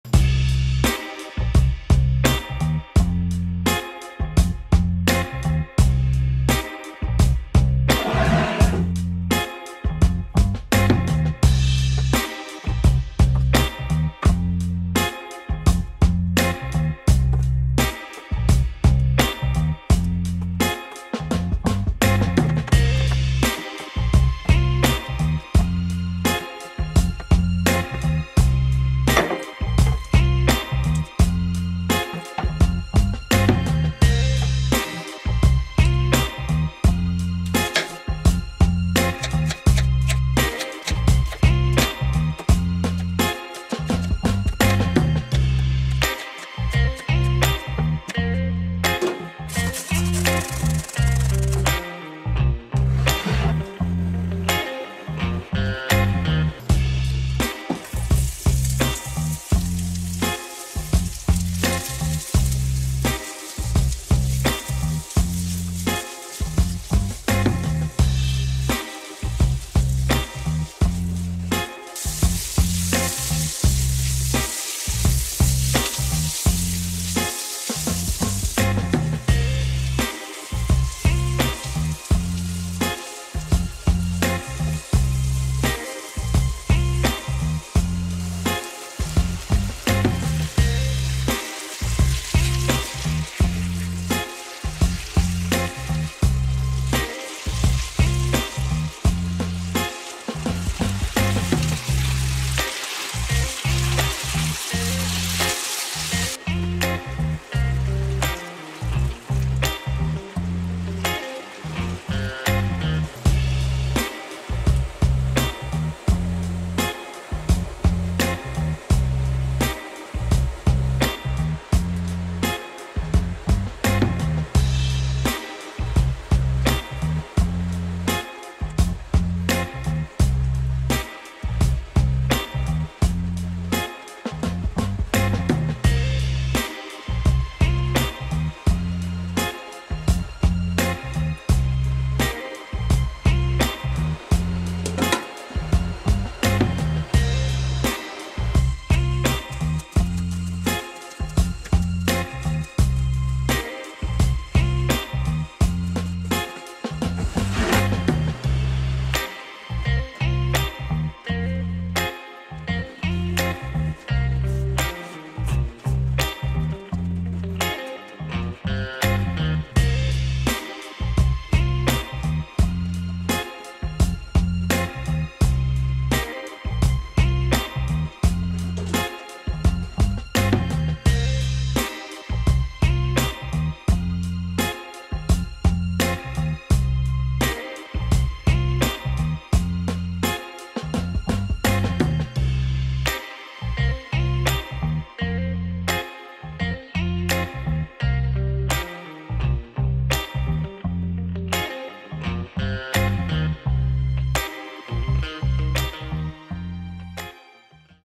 Iți trebuie doar 3 Ingrediente! Când nu știu ce să gătesc la cină, gătesc această mâncare simplă.
Rețeta de cină rapidă cu pui la tigaie.
O mâncare foarte apetisantă, aromată, ușoară și foarte delicioasă.
Dacă îți dorești un prânz delicios, atunci prepară acestă rețetă cu pui!

Ingrediente :
Piept de pui - 1 buc.
Ceapă - 2 buc.
Morcov - 1 buc.
Smântână dulce - 400 ml.
Sare / Piper

Dozus Cook vă dorește Poftă bună!